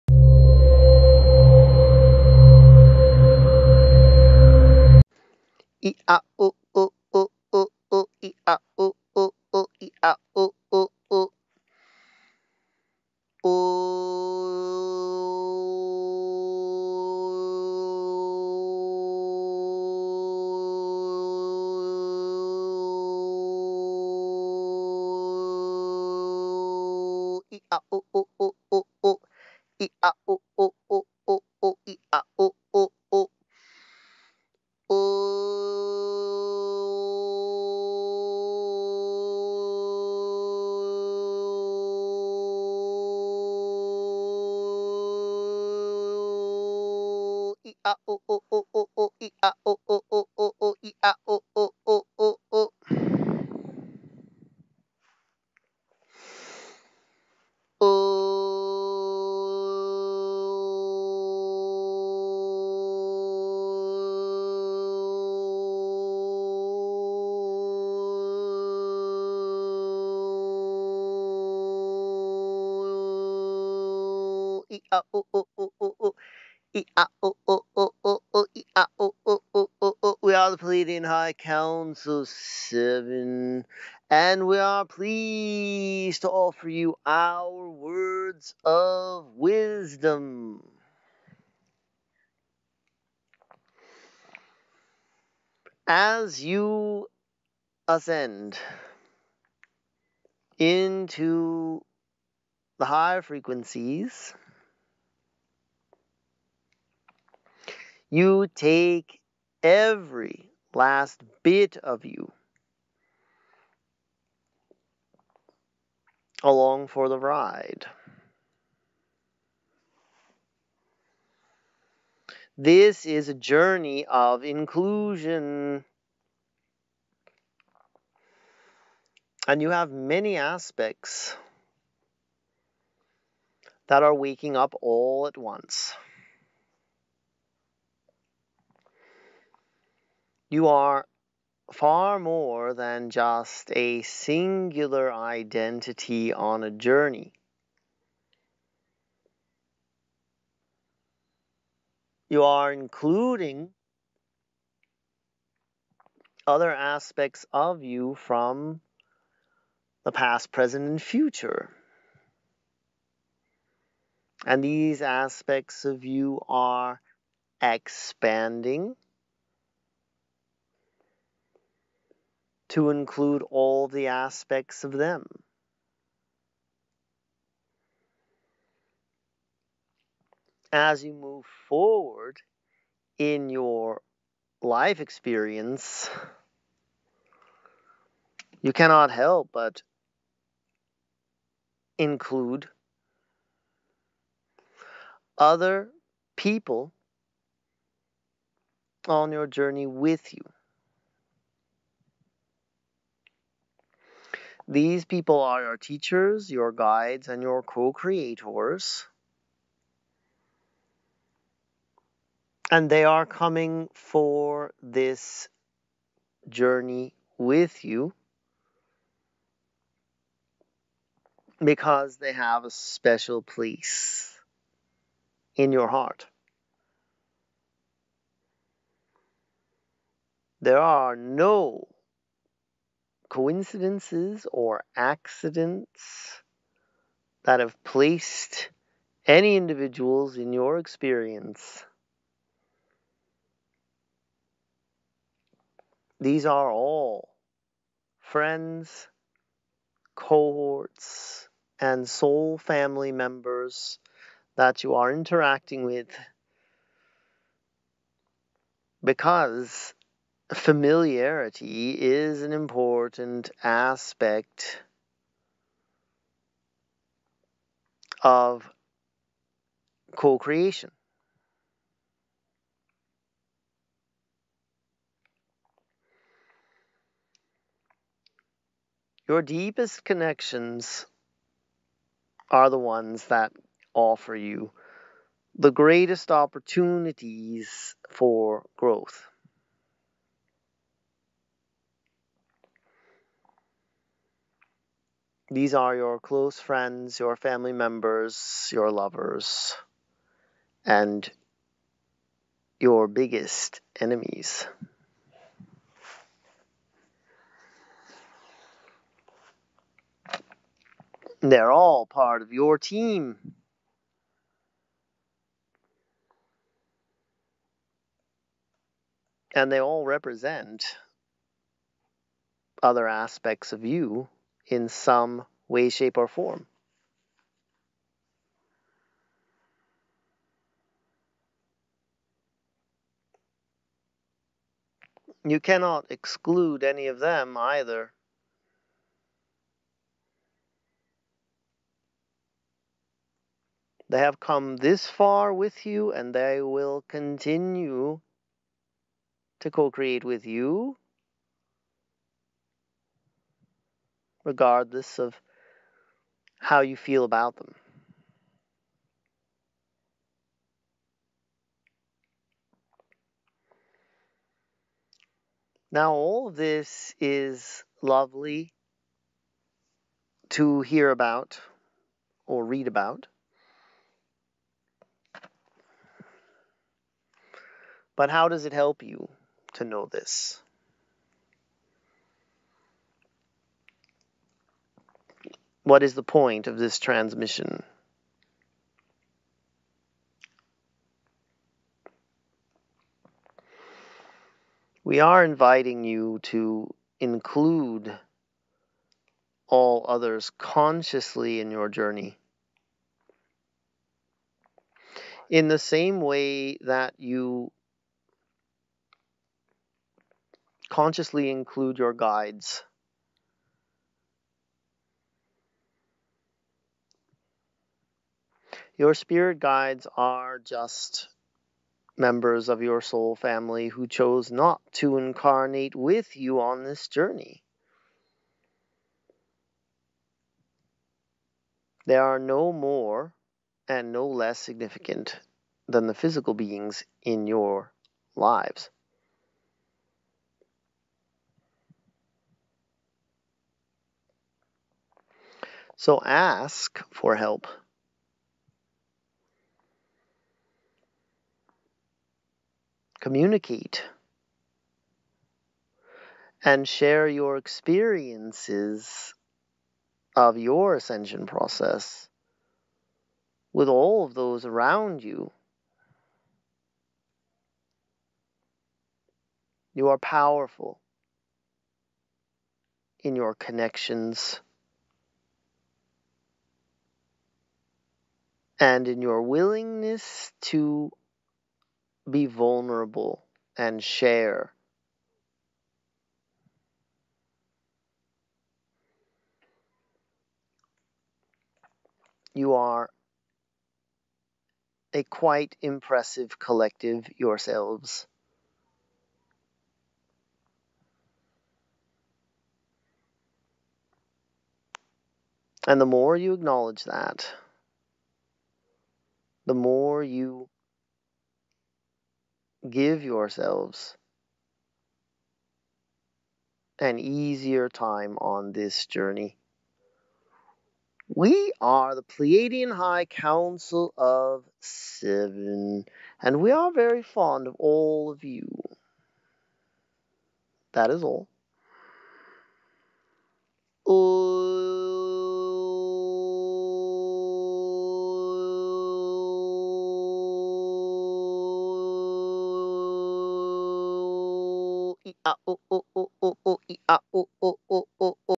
Your Co-Creators in Ascension ∞The Pleiadian High Council of Seven, Channeled by Daniel Scranton
“We are the Pleiadian High Council of Seven, and we are pleased to offer you our words of wisdom.

As you ascend into the higher frequencies, you take every last bit of you along for the ride. This is a journey of inclusion, and you have many aspects that are waking up all at once. You are far more than just a singular identity on a journey. You are including other aspects of you from the past, present, and future, and these aspects of you are expanding to include all the aspects of them.

As you move forward in your life experience, you cannot help but include other people on your journey with you. These people are your teachers, your guides, and your co-creators, and they are coming for this journey with you because they have a special place in your heart. There are no coincidences or accidents that have placed any individuals in your experience.

These are all friends, cohorts, and soul family members that you are interacting with because familiarity is an important aspect of co-creation. Your deepest connections are the ones that offer you the greatest opportunities for growth. These are your close friends, your family members, your lovers, and your biggest enemies. They are all a part of your team, and they all represent other aspects of you in some way, shape, or form.

You cannot exclude any of them either. They have come this far with you, and they will continue to co-create with you, regardless of how you feel about them. Now all of this is lovely to hear about, or read about, but how does it help you to know this? What is the point of this transmission?

We are inviting you to include all others consciously in your journey, in the same way that you consciously include your guides. Your spirit guides are just members of your soul family who chose not to incarnate with you on this journey. They are no more and no less significant than the physical beings in your lives.

So ask for help, communicate, and share your experiences of your ascension process with all of those around you. You are powerful in your connections and in your willingness to be vulnerable and share. You are a quite impressive collective yourselves, and the more you acknowledge that, the more you give yourselves an easier time on this journey.

We are the Pleiadian High Council of Seven, and we are very fond of all of you. That is all.”